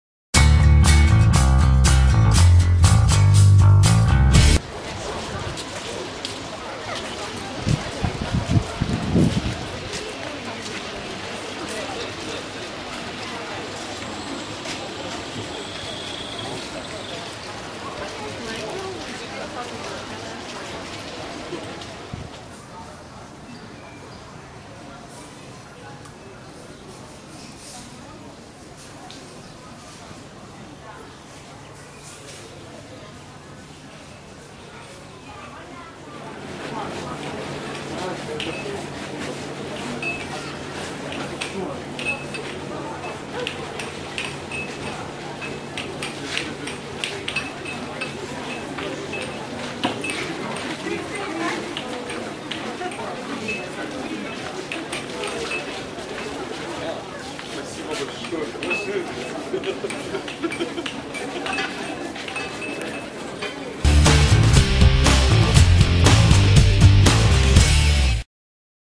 Дом Волошина. Коктебель 2011
12 июля 2011. Дом поэта Макса Волошина в Коктебеле.
"Крымский репортаж"